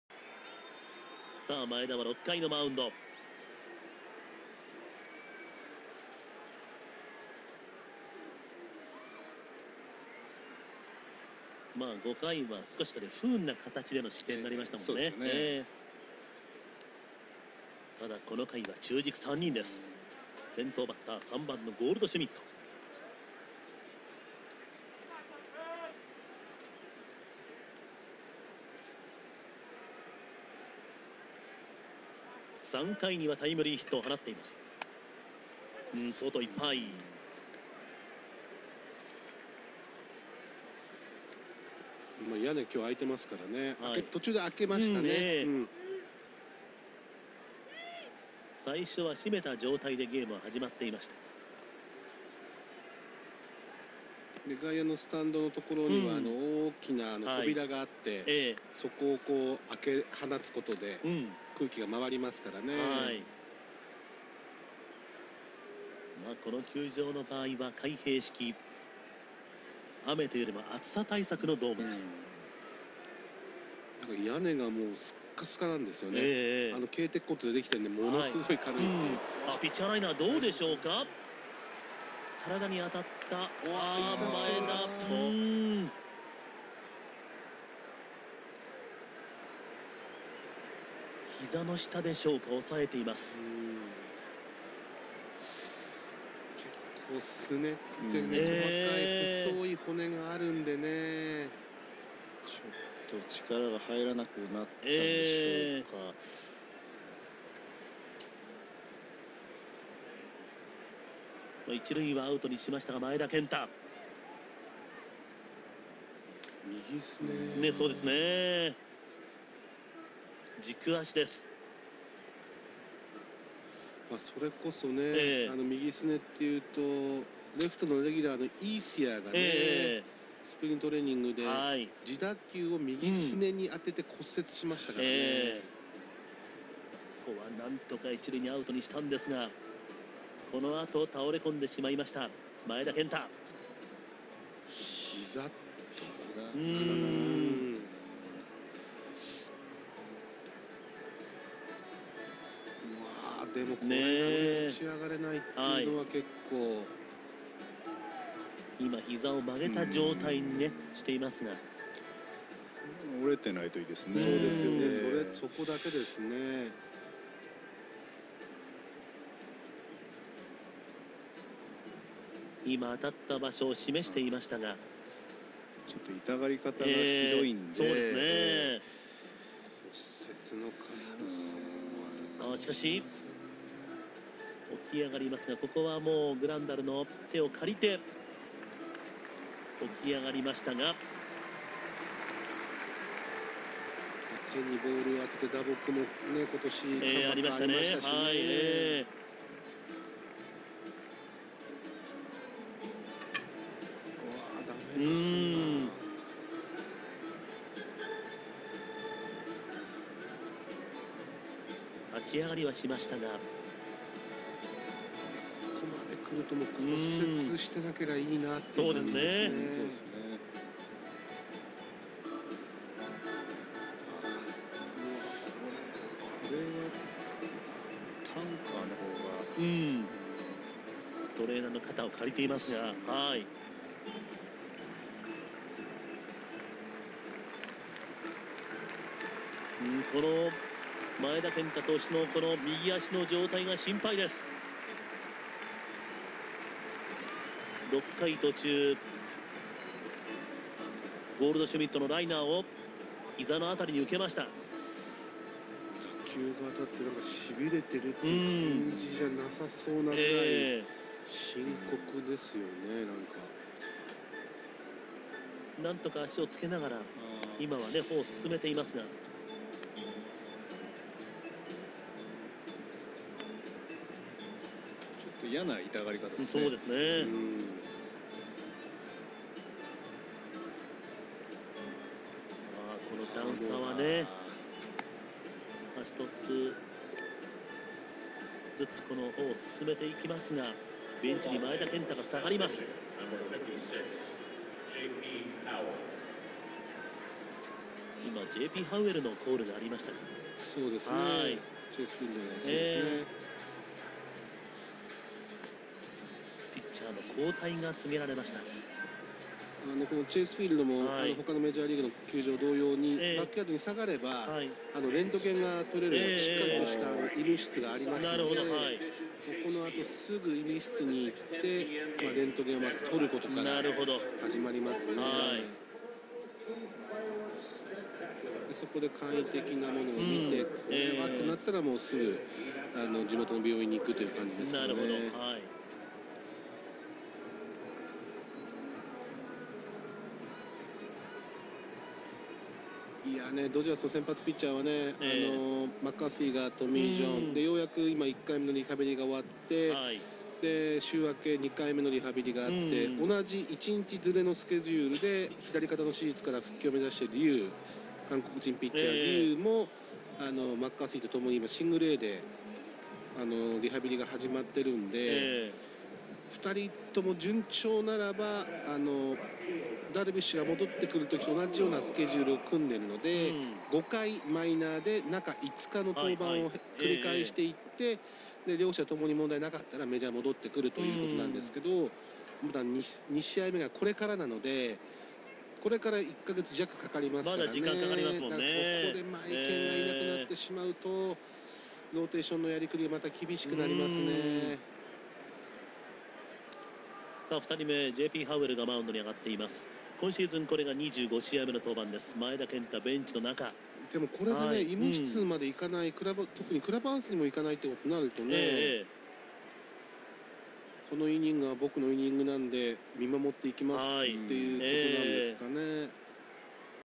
【マエケン６勝目も】 前田健太、右足に打球直撃で緊急降板(2016.6.14現地)
ドジャース　７－４　ダイヤモンドバックス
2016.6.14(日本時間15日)
前田健太投手はその後のＸ線検査の結果、骨には異常はなく、打撲と診断との情報。